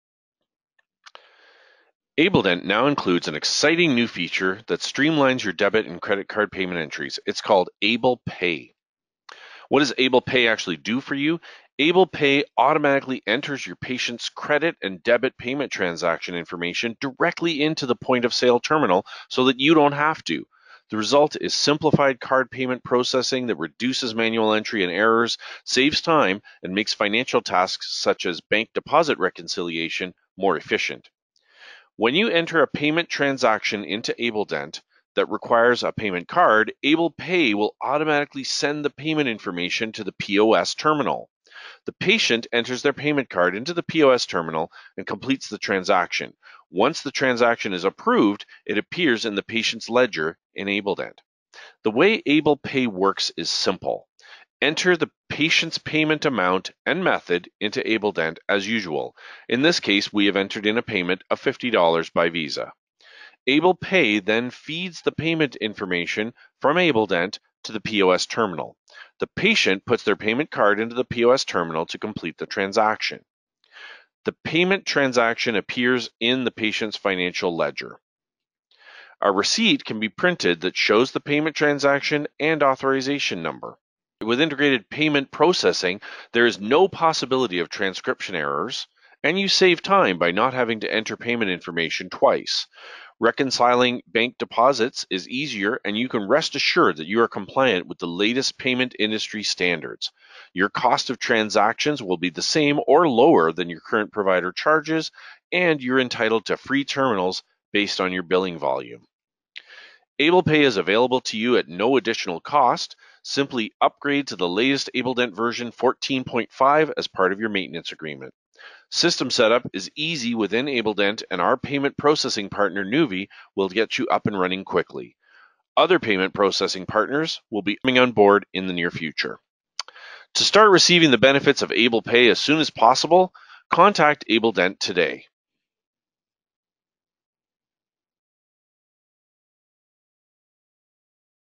Introducing ABEL Pay
Integrated Payment Card Processing Designed Specifically for ABELDent Users